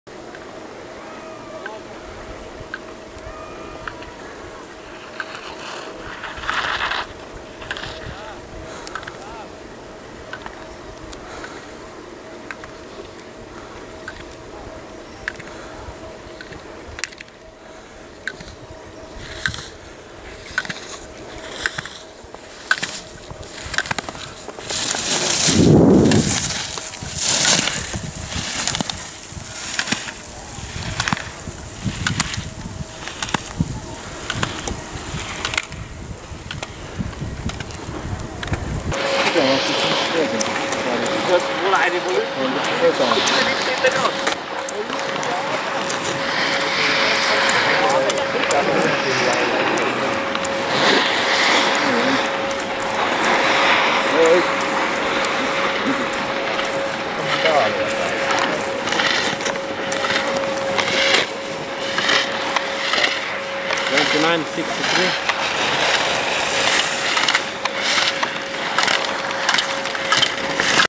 Ennen ja nyt
Sama pystyjyrkkä rinne, sama laskija. Välissä kolme vuotta. Mukava huomata kehittyneensä.